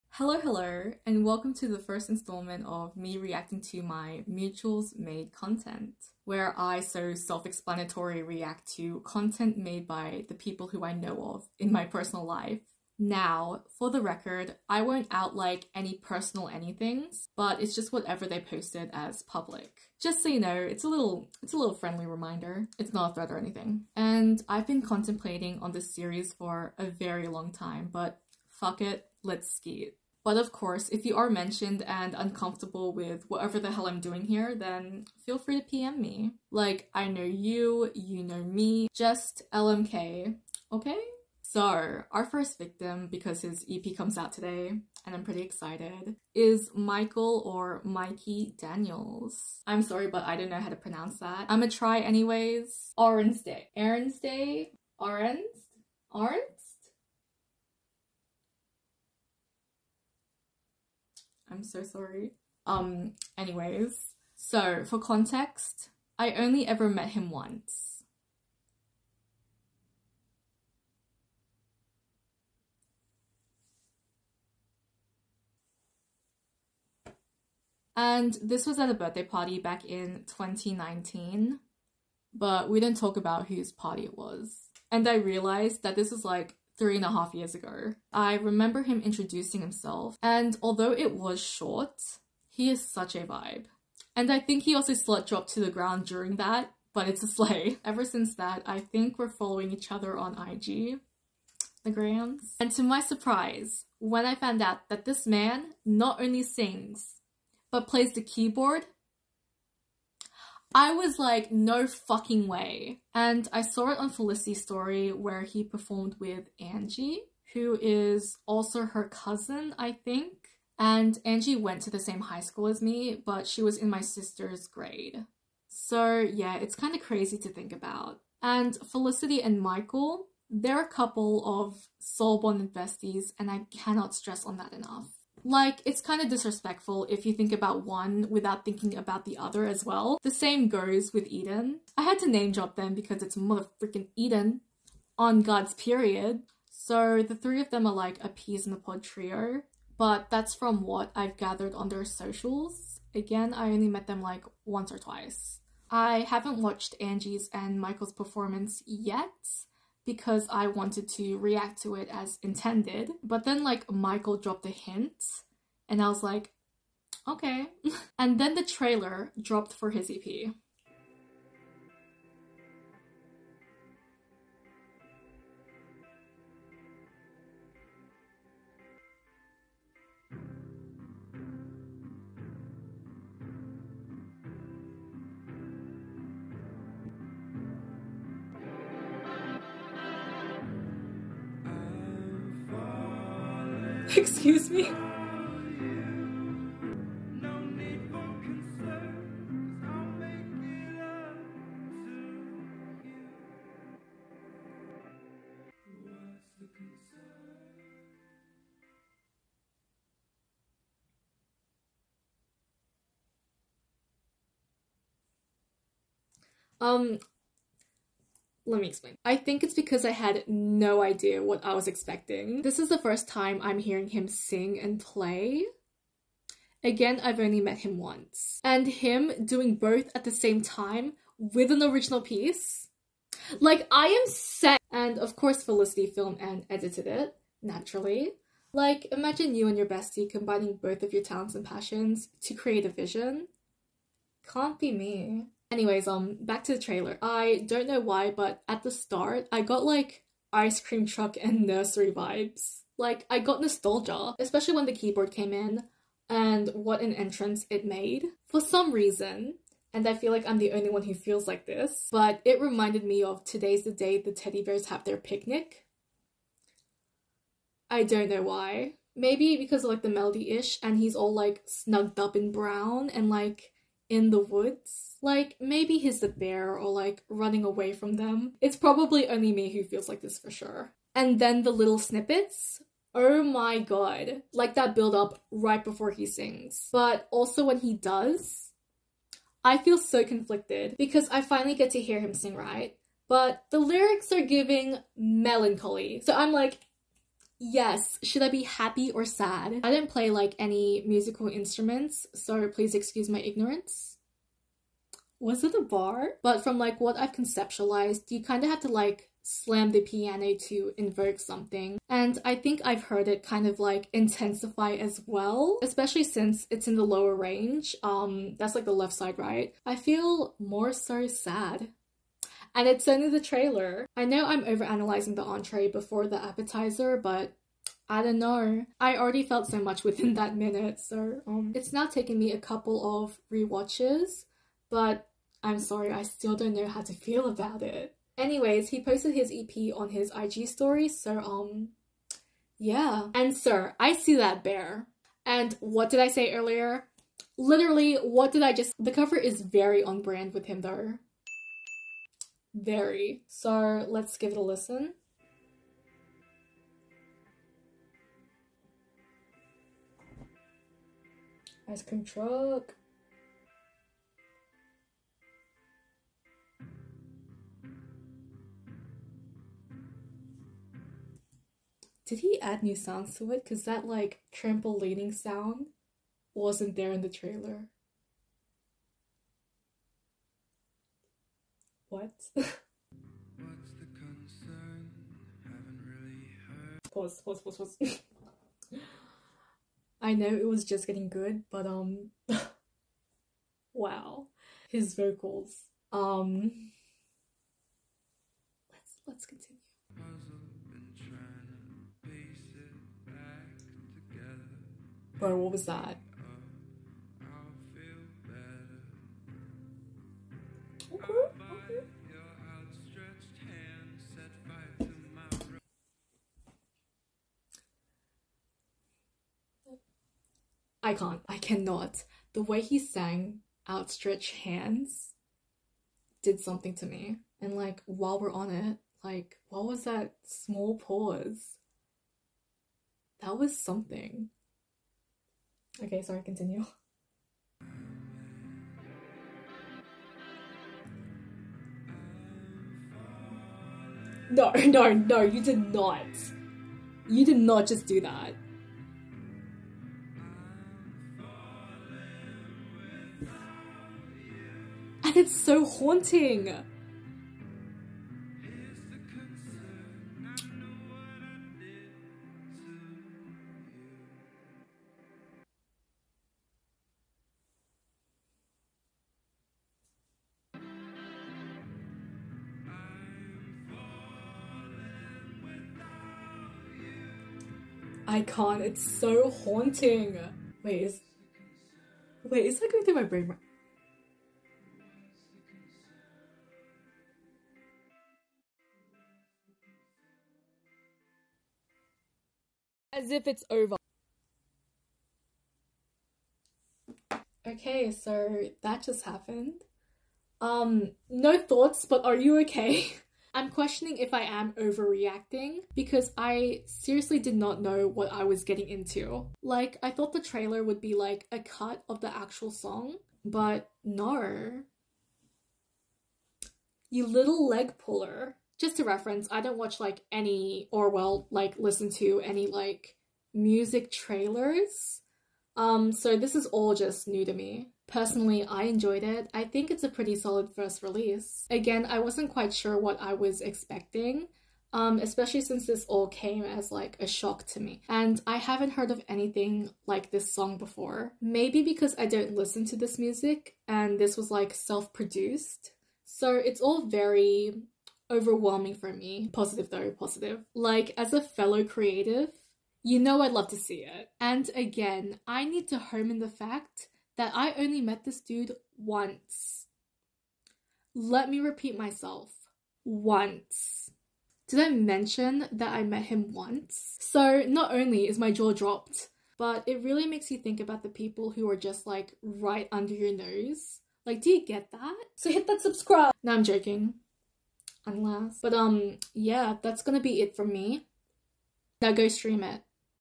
[REUPLOAD] Reacting to MY 'mutuals made' content
Ooft-- so this is a reupload from my priv IG lolol
This defs wasnt intended for the YT experience, but its one of my first videos that made me reconsider this whole thing bc yall are hella inspiring xx

Thank you to ✨ALL✨ la peeps whove given me the love n support for this!!
Moving forward, I hope I could do the same for yall one day 🩵🩵
~ Love Odessa

*NOTE: Also sorry for the quality again, ik its atrocious!!*
It does NOT do this man justice, but I cant find the OG files to fix ittt 😭😭

Also yall are already guaranteed a pt.2 for this - idk when, but its coming (n Ill promise itll be better formatted)